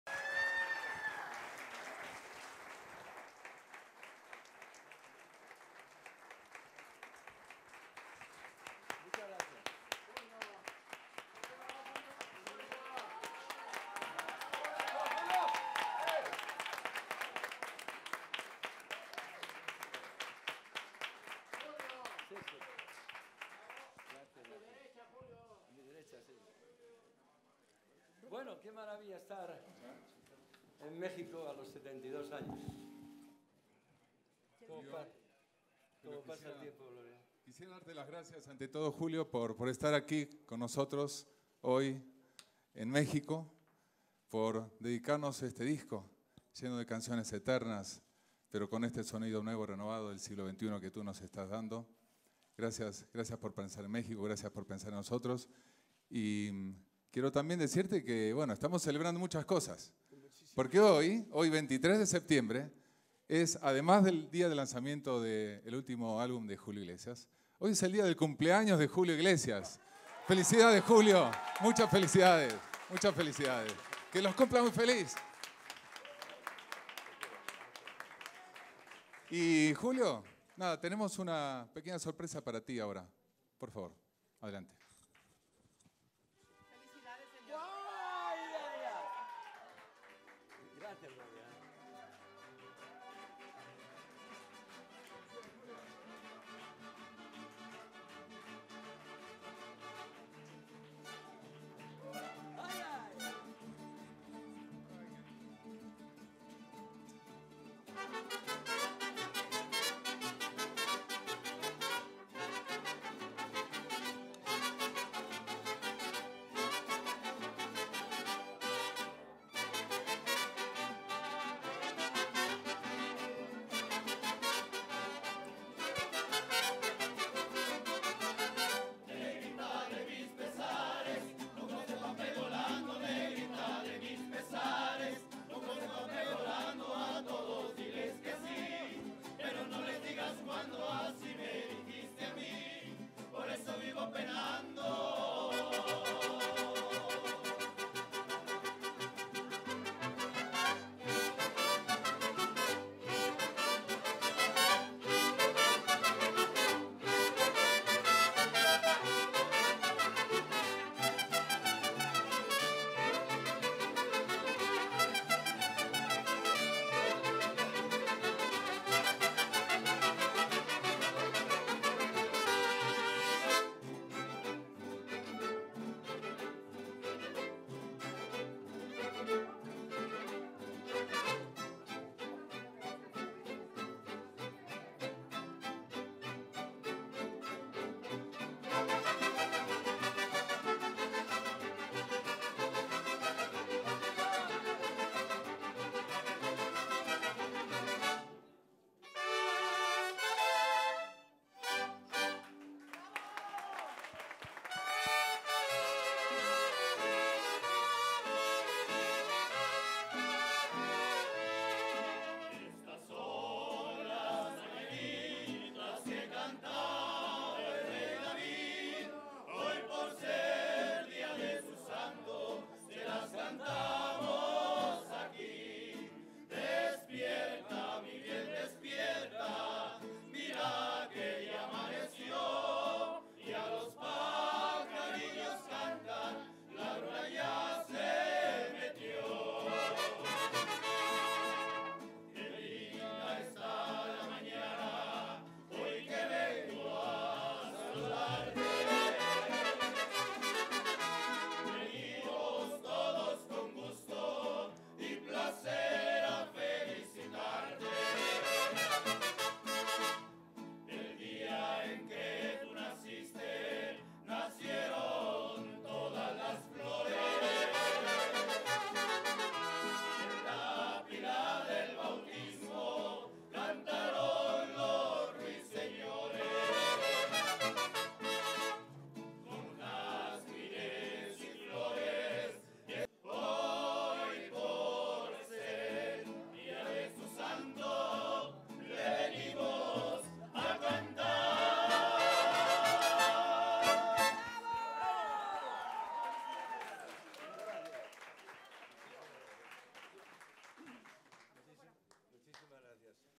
Tremenda sorpresa en el dia del cumpleaños 72 de Julio Iglesias
Julio Iglesias en su cumpleaños 72, uno de los grandes cantautores festejo por todo lo alto en Mexico